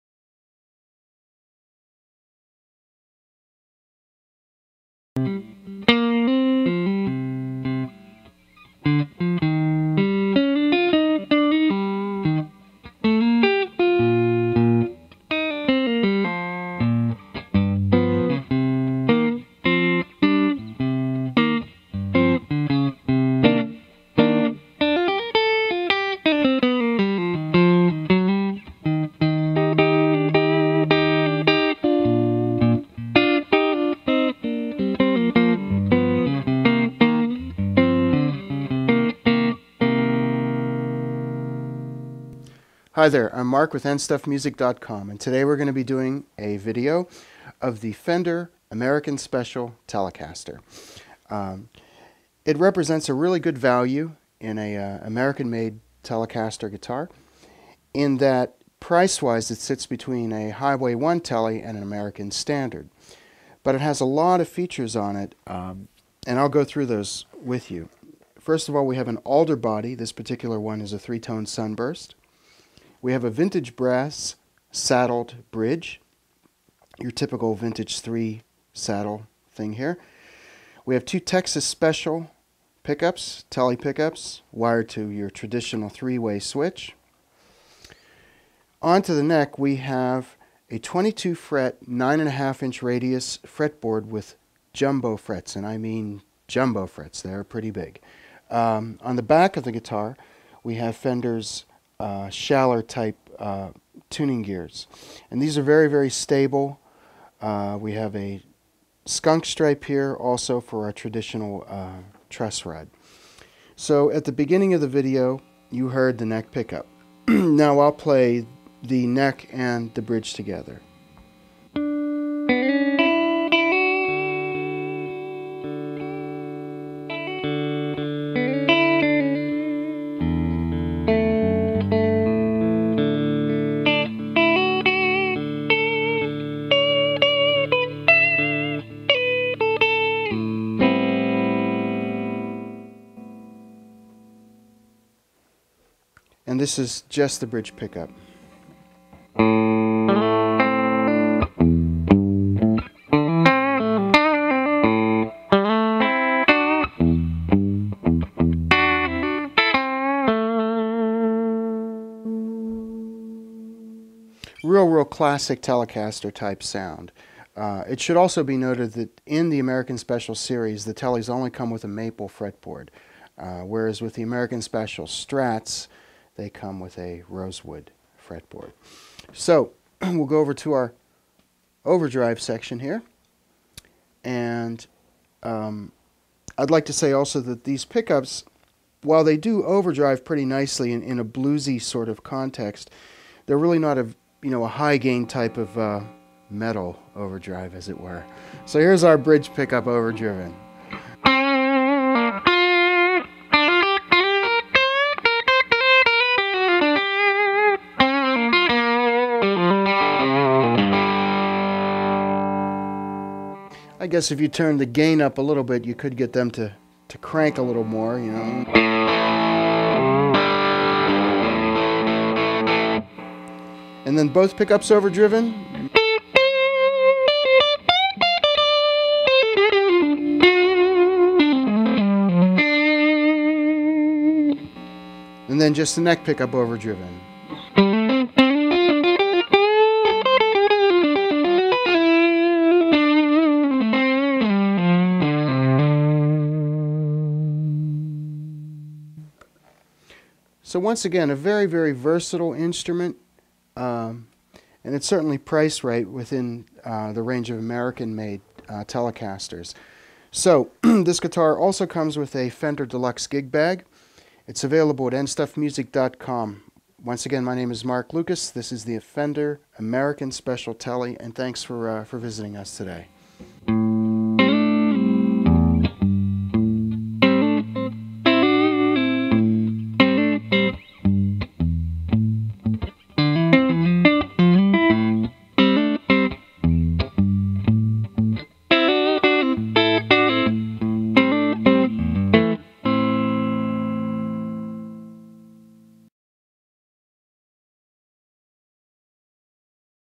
Fender American Special Telecaster
Fender introduces the American Special Series Telecaster. Everything you want in an affordable U.S.-made guitar is within your reach.

Features include an alder body with gloss urethane finish, 9.5"-radius maple neck, jumbo frets, Texas SpecialTM pickups, and vintage-style three-brass-saddle strings-through-body Telecaster bridge. The American Special Series - guitars for the times.

SPECIFICATIONS

Model Name:
 American Special Telecaster®

Model Number:
 011-5802-(Color #)

Series:
 American Special Series

Colors:
 (300) 3-Color Sunburst,
(305) Olympic White,
(Gloss Urethane Finish)

Body:
 Alder

Neck:
 Maple, Modern "C" Shape,
(Gloss Urethane Finish)

Fingerboard:
 Maple, 9.5" Radius (241 mm)

No. of Frets:
 22 Jumbo Frets

Pickups:
 1 Texas SpecialTM Tele® Pickup (Bridge),
1 Texas SpecialTM Tele® Pickup (Neck)

Controls:
 Master Volume,
Master GreasebucketTM Tone Circuit, (Rolls Off Highs without Adding Bass)

Pickup Switching:
 3-Position Blade:
Position 1. Bridge Pickup
Position 2. Bridge and Neck Pickup
Position 3. Neck Pickup

Bridge:
 Original Vintage Tele Bridge with 3 Brass Saddles

Machine Heads:
 Fender® Standard Cast/Sealed Tuning Machines

Hardware:
 Chrome

Pickguard:
 3-Ply Black/White/Black

Scale Length:
 25.5" (648 mm)

Width at Nut:
 1.6875" (43 mm)

Unique Features:
 GreasebucketTM Tone Circuit,
3 Texas SpecialTM Pickups,
Large '70s Style Decal,
Black Pickup Covers and Plastic Parts,
Brass Bridge Saddles,
Black Dot Position Inlays,
"Barrel" Switch Tip

Strings:
 Fender USA, Nickel Plated Steel,
Gauges: (.009, .011, .016, .024w, .032, .042)

Accessories:
 Deluxe Gig Bag, Polishing Cloth

INCLUDES DELUXE GIG BAG .

468 Freeport Road
Pittsburgh PA 15238